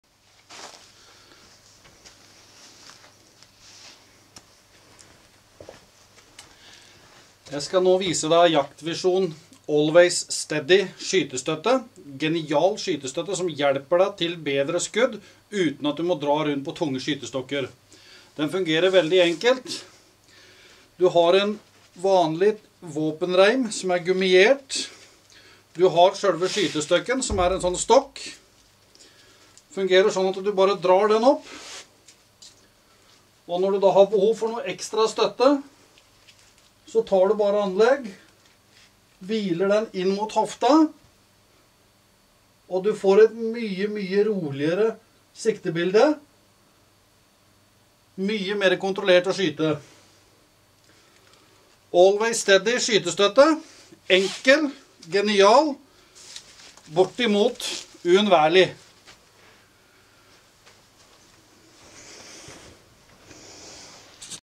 Jaktvision Always Steady integrert skytestøtte/ våpenrem.
En genial oppfinnelse som gjør at du kan avgi mer stabile og presise skudd ved fyssisk anstrengelse når du ikke har mulighet til å ta anlegg mot annet. Denne vil ved stående skyting, bidra vesentlig til å redusere risikoen for skadeskyting, da du vil få et roligere siktebilde ved skuddavgivning.